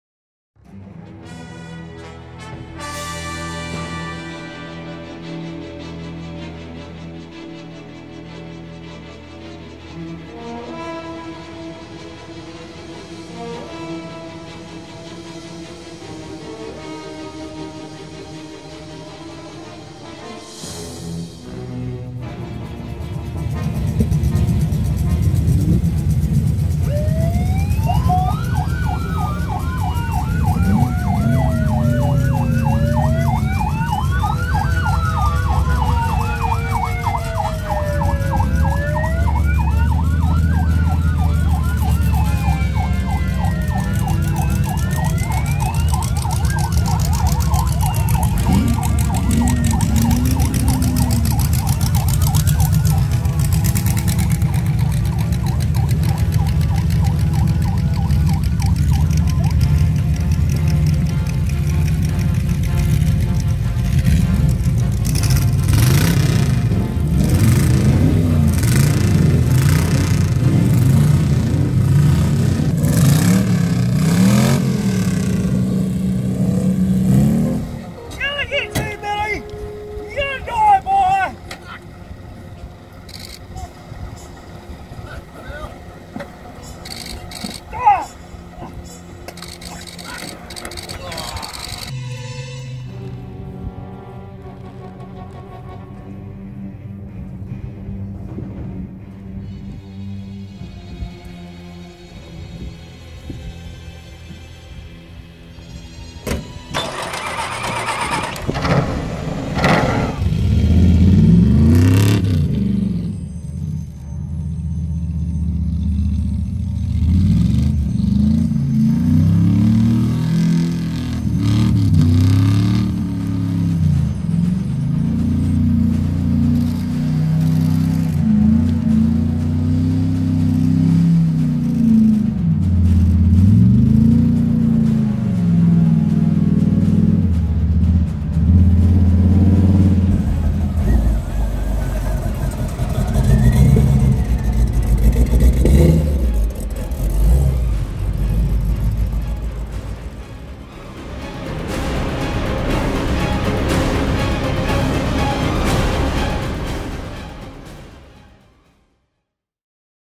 Little Max
Fan movie filmed on the next day after Weekend with Max 2023 at Narrandera,NSW